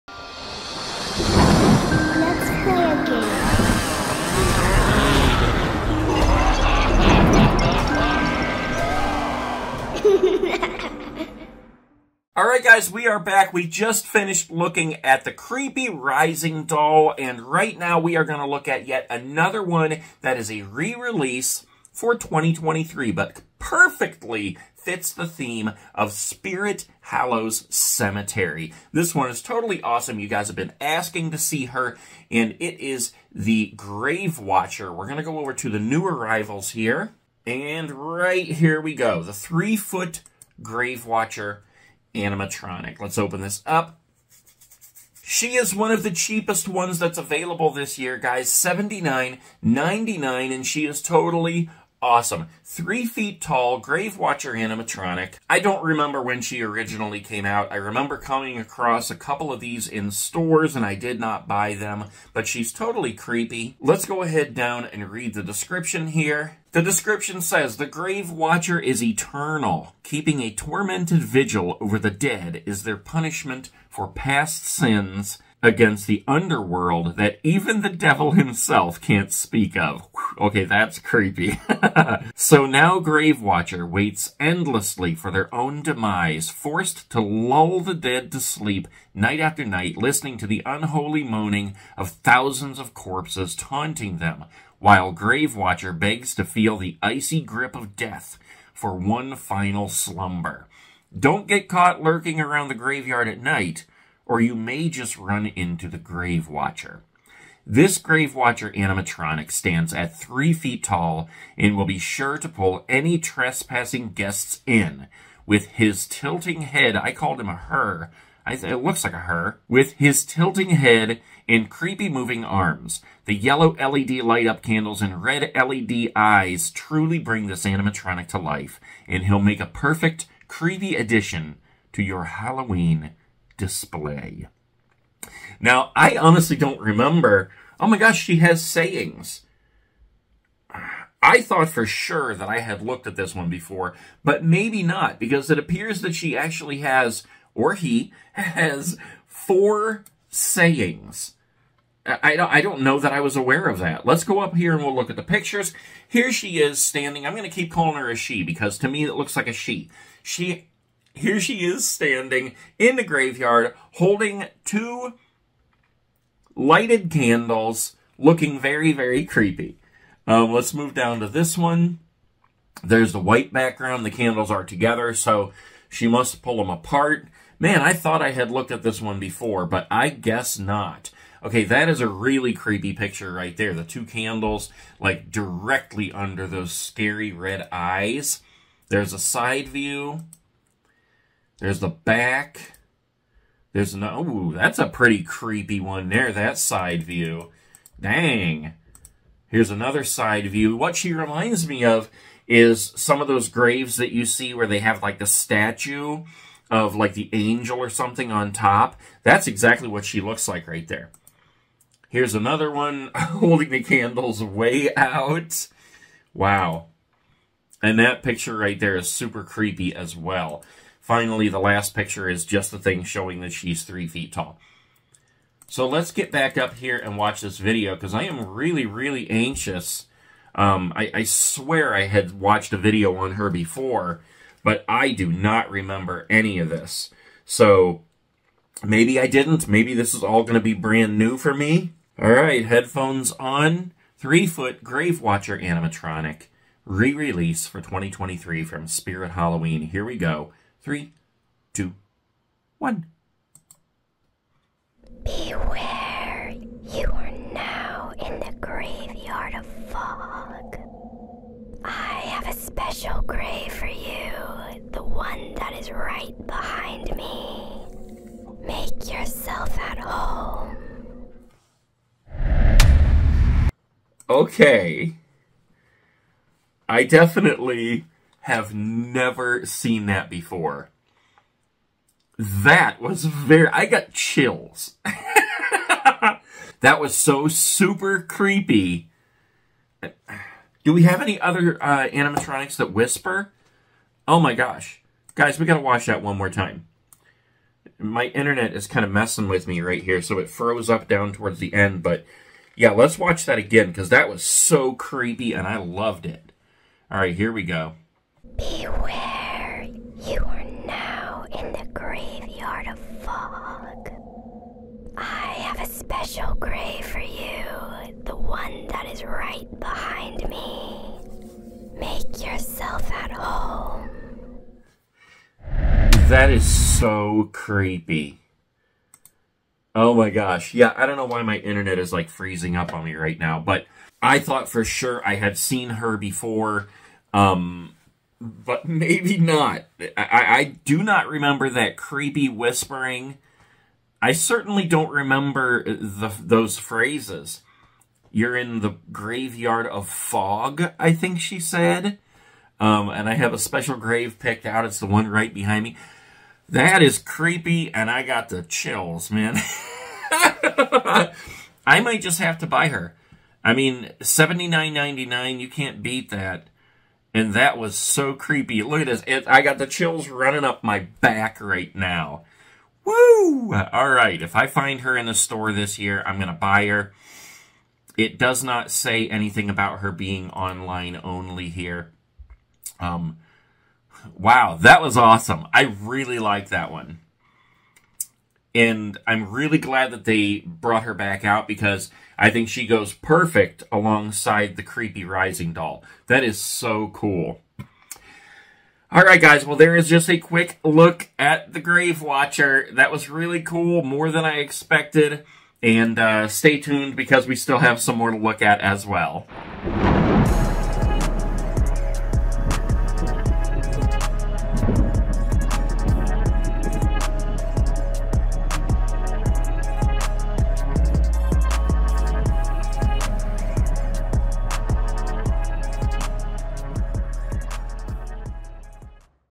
SPIRIT HALLOWEEN 2023 GRAVE WATCHER ANIMATRONIC REVEAL REACTION SNEAK PEEK SPIRIT HALLOWS CEMETERY
CREEEEEEEEEEEEEPY!!! Today we are checking out the super creepy GRAVE WATCHER animatronic from spirit halloween 2023

SEND FAN MAIL TO NATE!
        That Nate Guy On YouTube